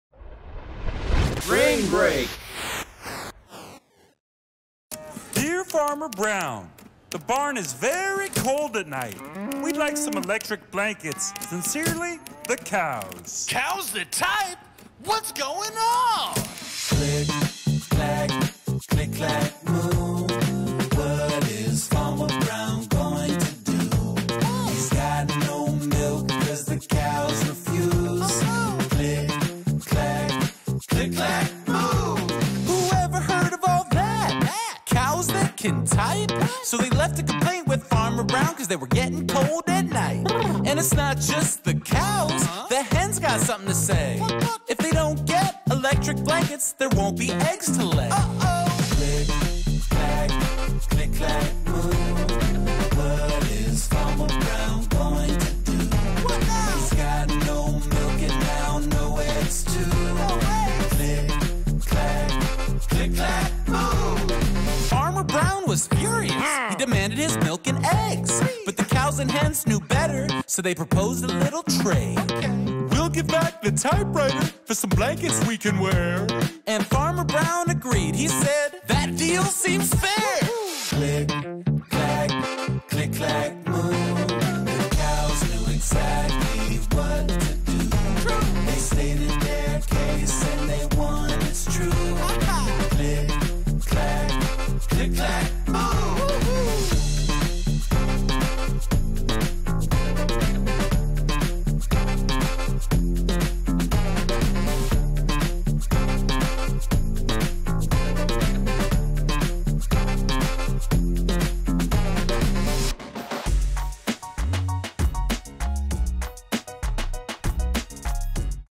Book Boys Brain Break: Click, Clack, Moo: Cows That Type
After reading the Book of the Week, Click, Clack, Moo: Cows That Type, written by Doreen Cronin and illustrated by Betsy Lewin, the Book Boys reenact the story of how some cold cows and hens hatch a plan to get warm blankets from Farmer Brown. Play the video for your class as a brain break between lessons or after recess or lunch!